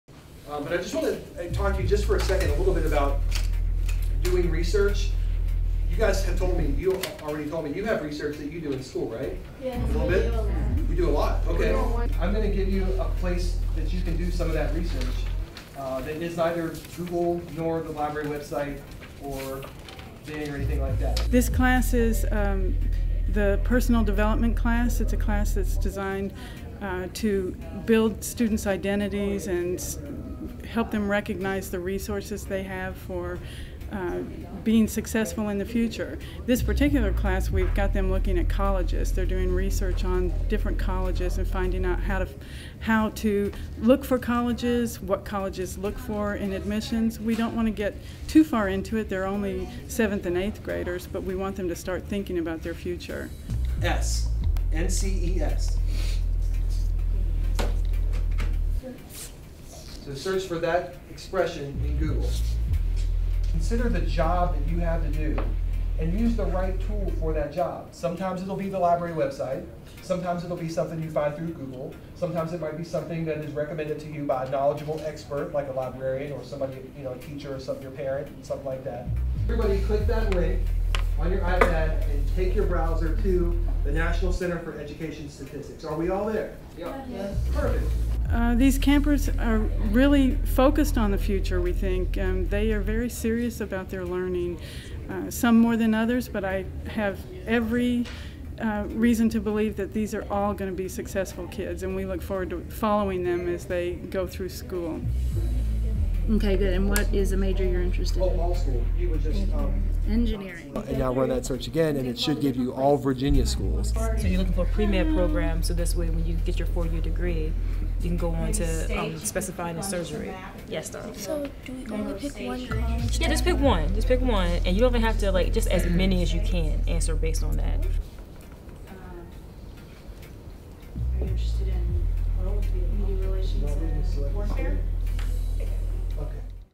Camp Launch: Envisioning the future at Swem
Participants in Camp Launch spent a few hours at Swem Library during their two-week stay at William & Mary.